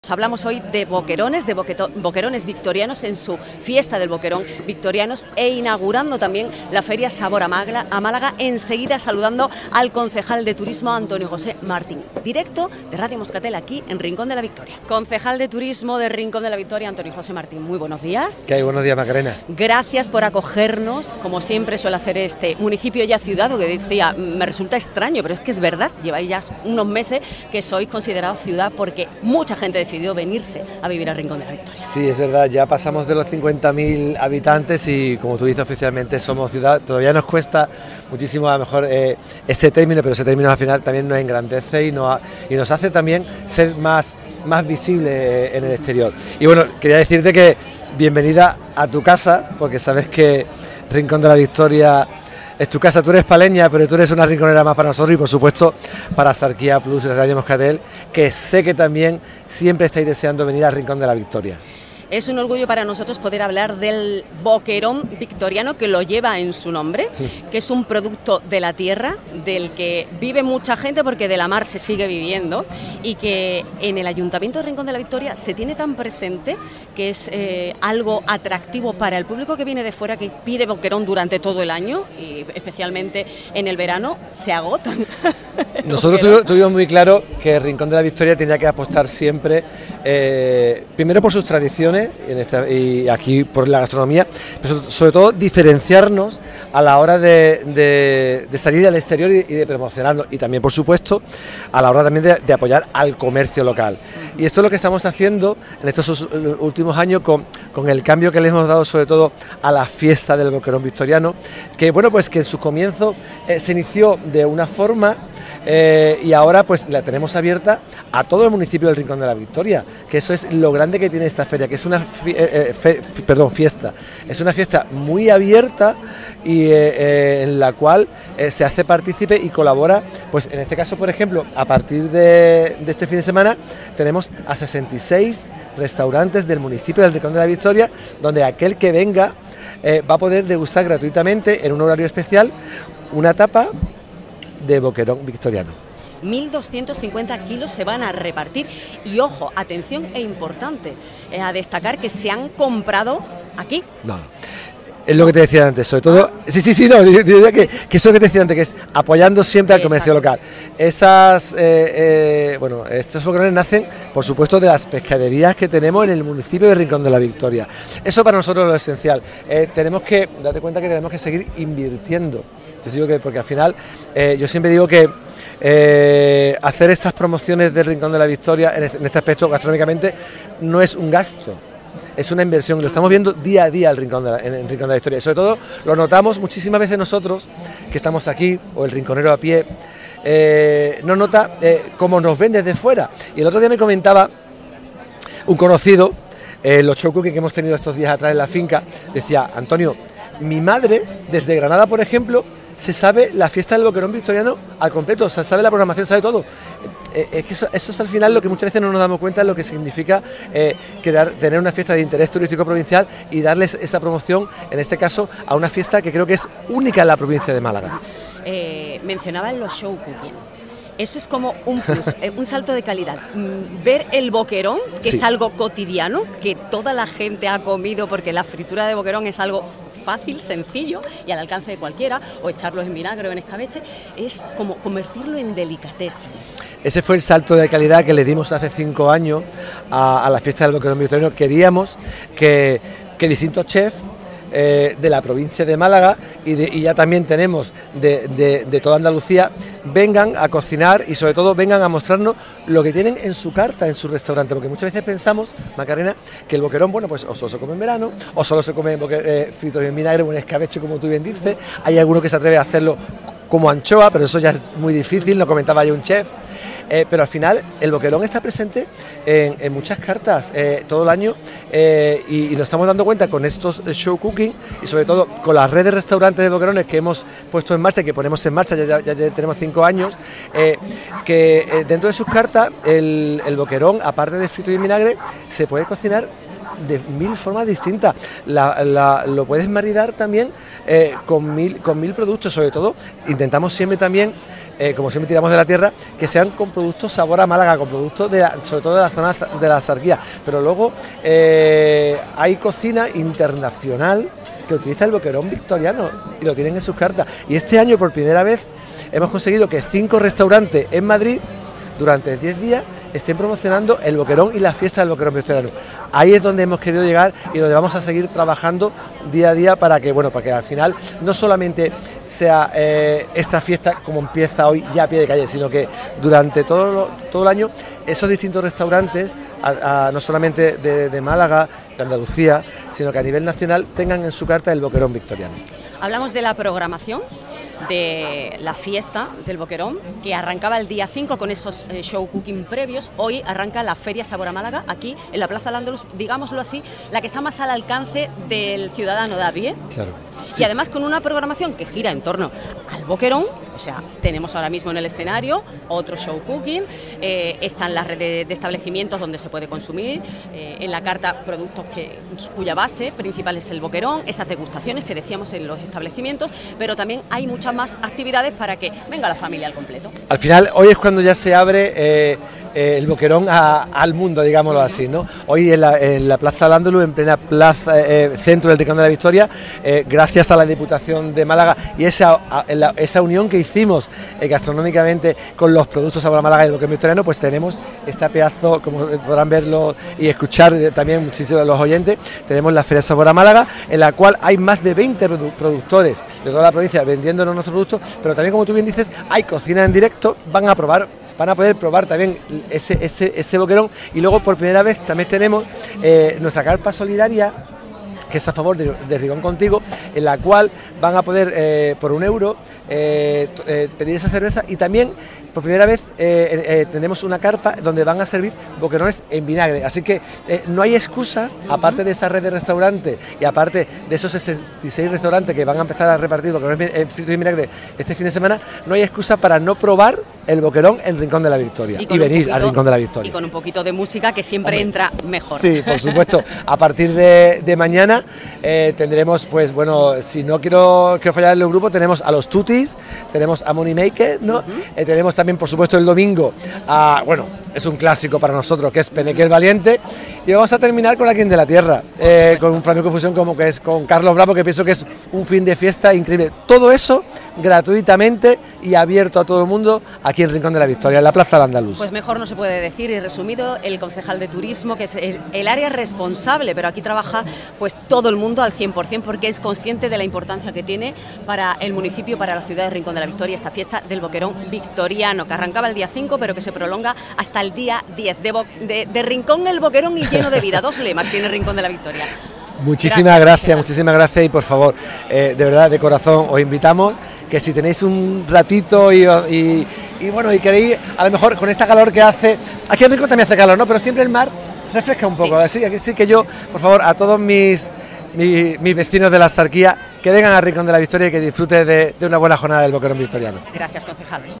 Antonio José Martin te invita a la Fiesta del Boquerón Victoriano 2023
Antonio José Martin te invita a la Fiesta del Boquerón Victoriano 2023, concejal de Turismo del Ayuntamiento de Rincón de la Victoria, te invita a la Fiesta del Boquerón Victoriano 2023